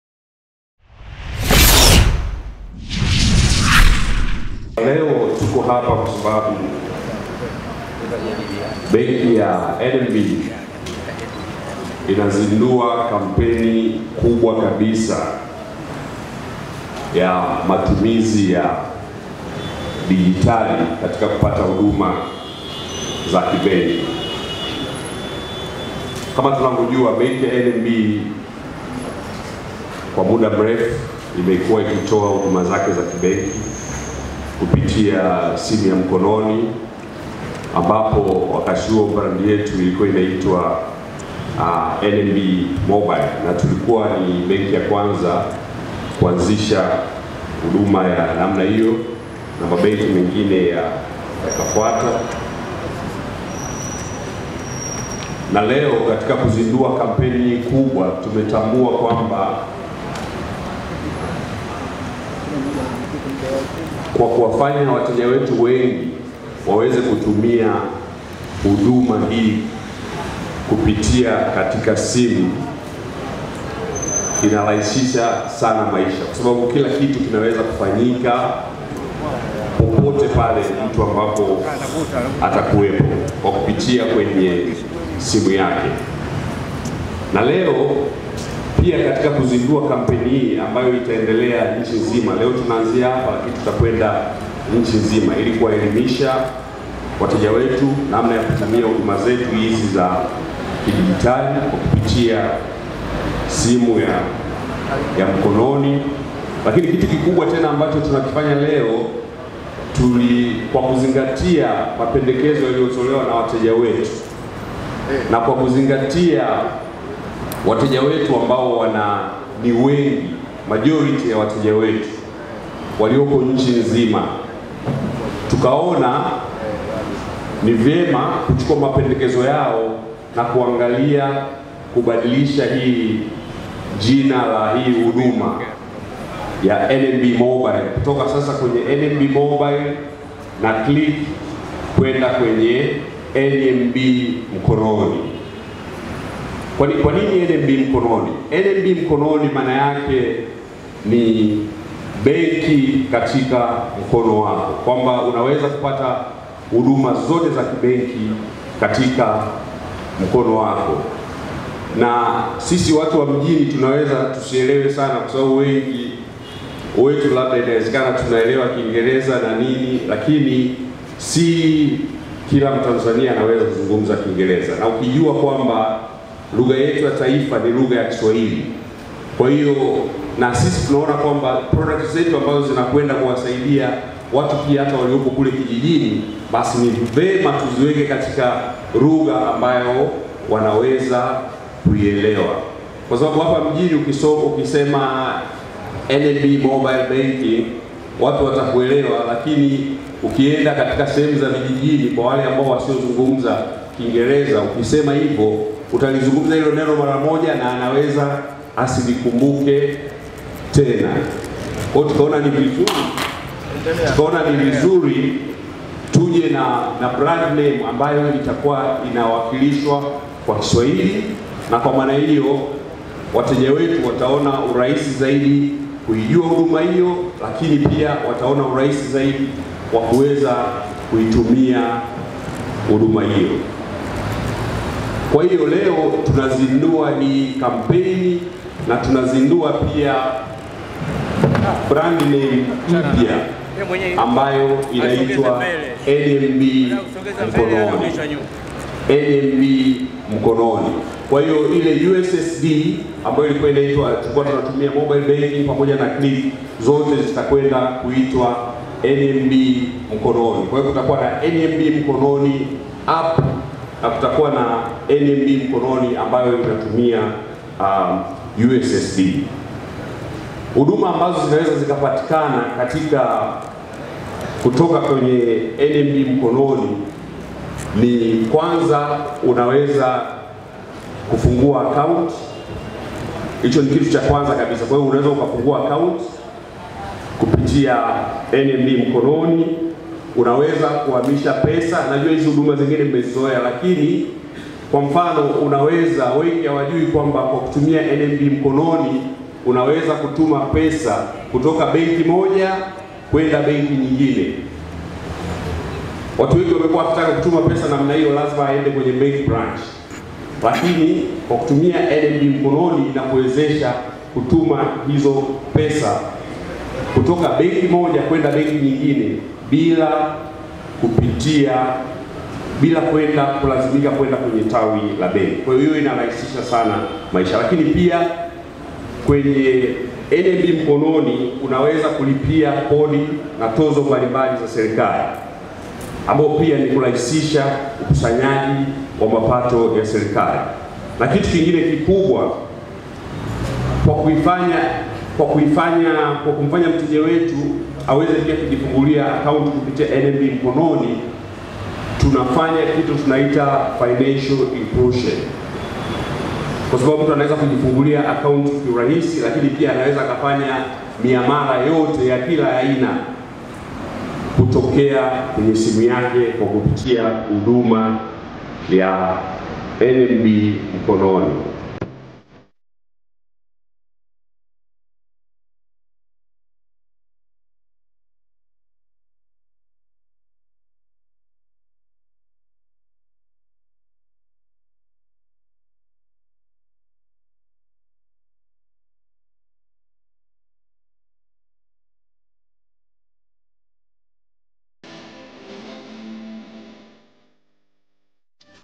GOOD NEWS: Kwa Wateja wa NMB na Huduma Mpya ya NMB Mkononi..
BENKI ya NMB imezindua kampeni Mpyaya kidijitali katika kupata huduma za kibenki ijulikanayo kama NMB Mkononi iliyotokana na kuunganishwa kwa huduma mbili za NMB MOBILE, KLIK lengo likiwa ni kuongeza ufanisi kwa watumiaji wa simu za mkononi.